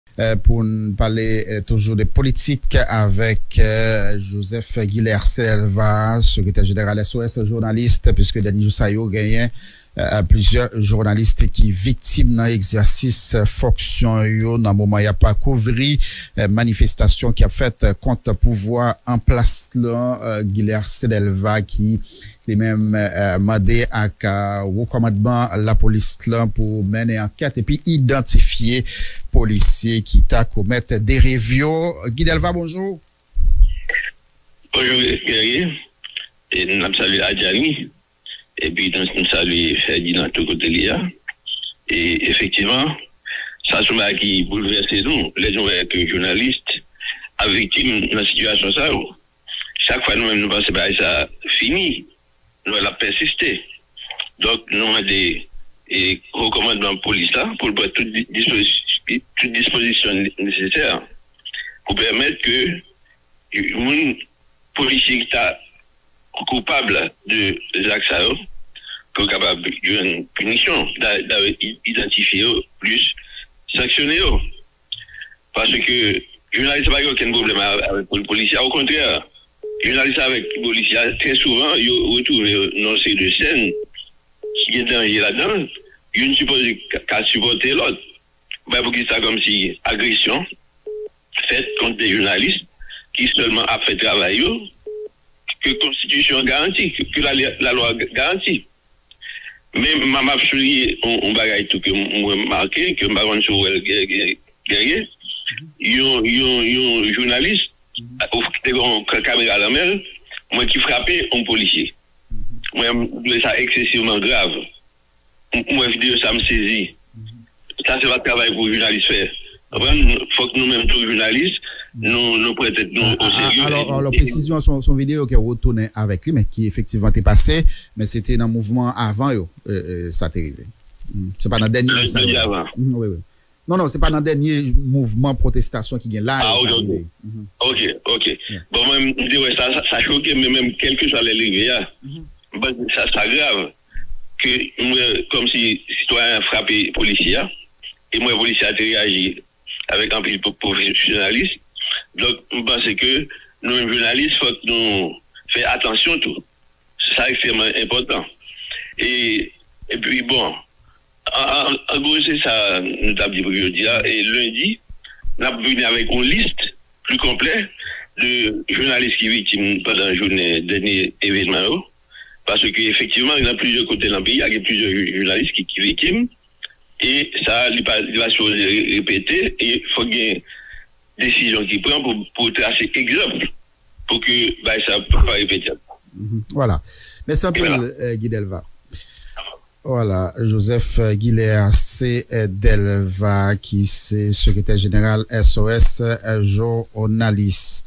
Joseph Guyler C. Delva kondane move tretman kèk jounalis sibi nan men kèk ajan polisye
Joseph Guyler C. Delva kondane move tretman kèk jounalis sibi nan men kèk ajan polisye nan dènye jou sa yo.